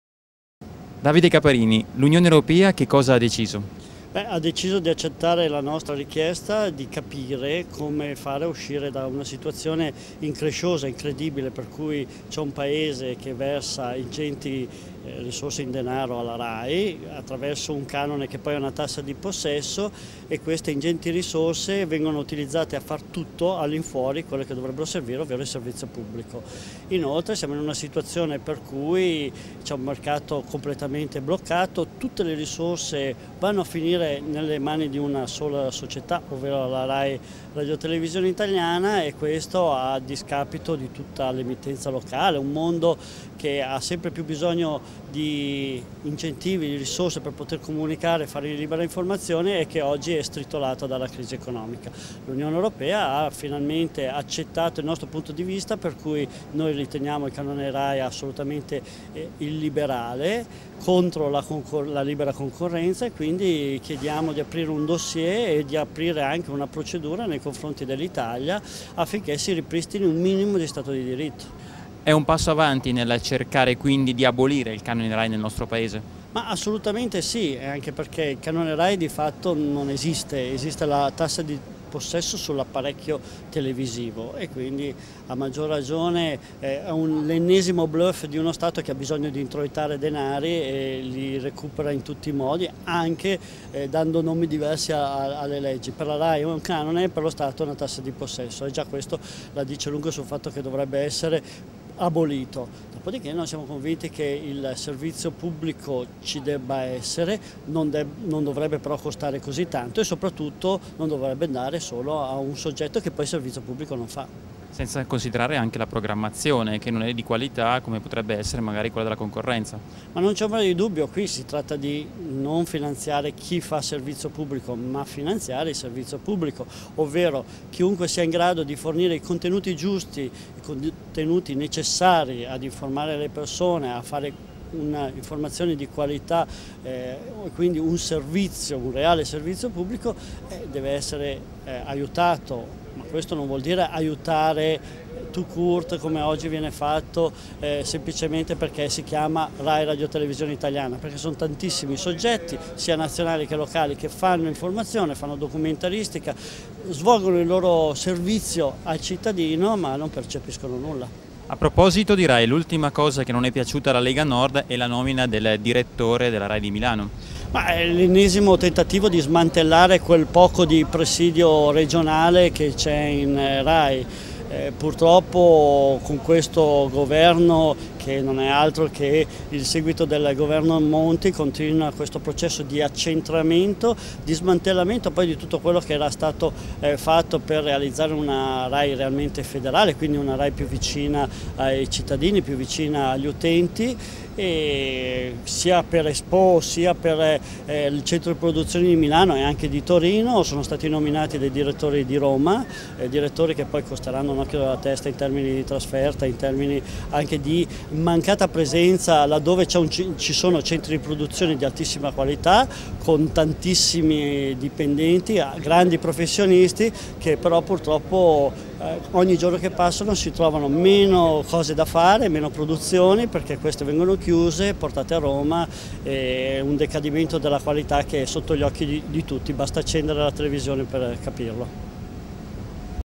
CANONE RAI: LEGA, DA BRUXELLES IL PRIMO STORICO SÌ AD ABOLIZIONE
(AGENPARL) - Roma, 06 set -  L'abolizione del Canone
Rai incassa il primo "storico sì" da parte di Bruxelles.Il
 Parlamento Europeo ha ufficialmente accolto e dichiarato
ammissibile la  Petizione per l'abolizione del Canone
Rai, chiedendo alla Commissione  UE di aprire  un'indagine
sui vari aspetti del problema.

La petizione per l'abolizion
e del canone, promossa dall'europarlamentare leghista
Mara Bizzotto e dal CLIRT (Comitato per la Libera Informazion
e  Radio Televisiva) di Marostica (VI), era stata depositata
a Bruxelles  nel dicembre 2012 supportata dalle firme
di migliaia di cittadini di  tutta Italia. In questi
mesi la Petizione, che chiede  espressamente l'abolizione
del canone e l'apertura di una procedura  d'infrazione
comunitaria contro l'Italia per violazione della libera
 concorrenza, è passata al vaglio della "Commissione
Petizioni"  dell'Europarlamento che ha dato il via libera
ufficiale  all'ammissibilità delle questioni sollevate
e ha portato all'avvio di  un'indagine da parte della
Commissione UE.